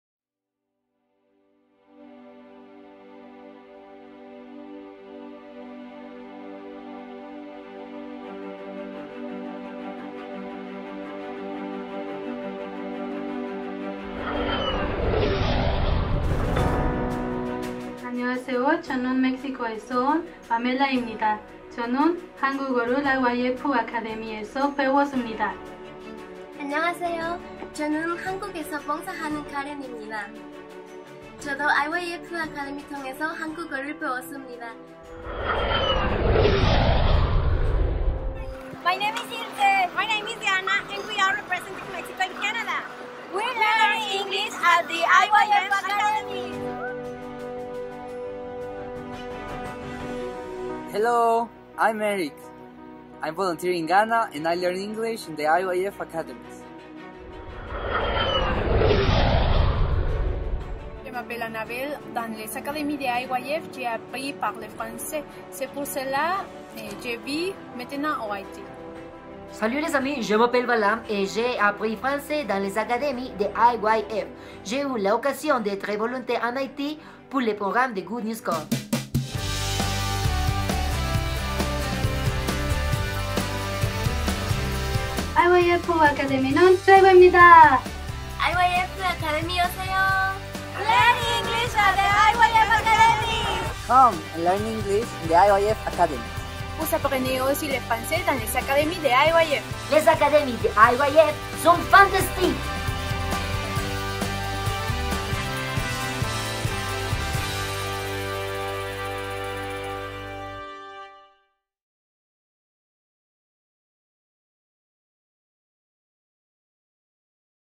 Academias de IYF
Es un video promocional de las Academias de Idiomas de IYF (Coreano, francés e inglés).